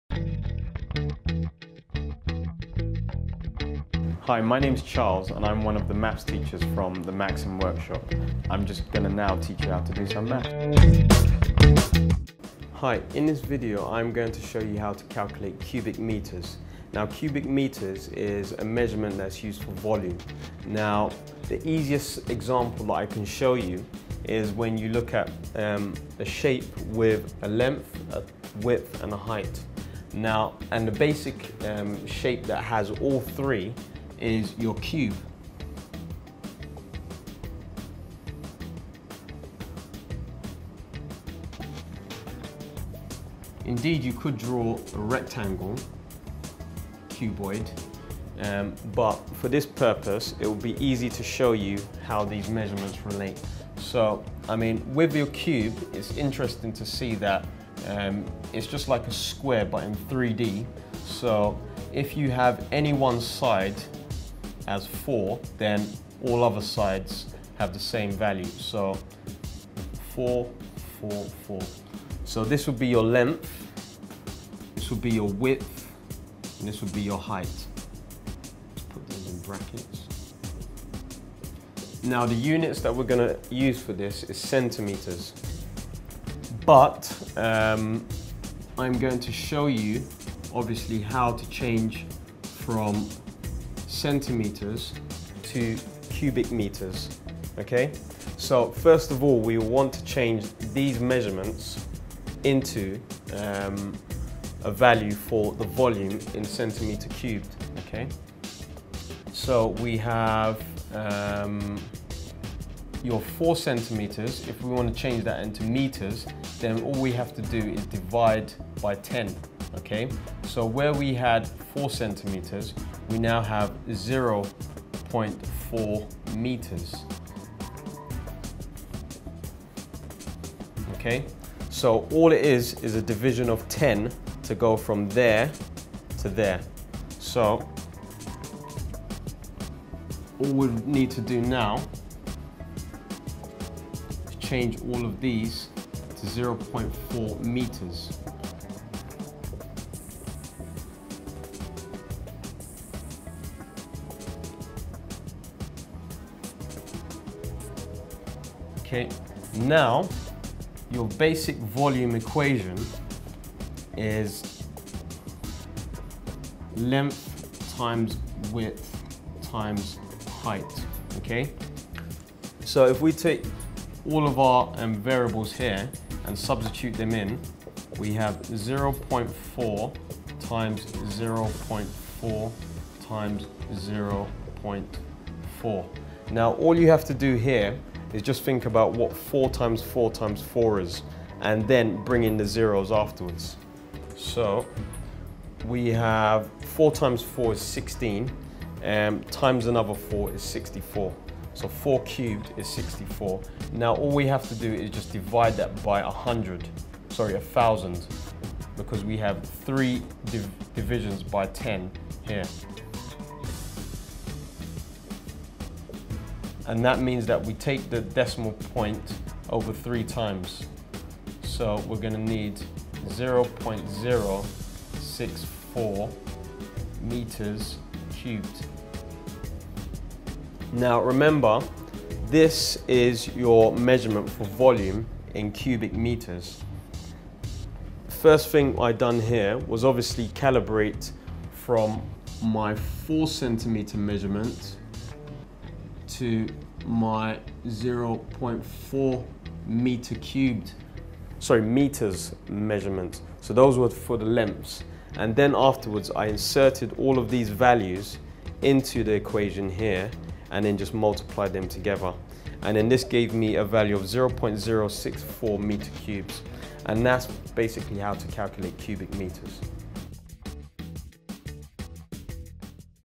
How To Compute Cubic Meters
This guide is a suitable time-saver that will enable you to get good at math. Watch our tutorial on How To Compute Cubic Meters from one of Videojug's professional experts.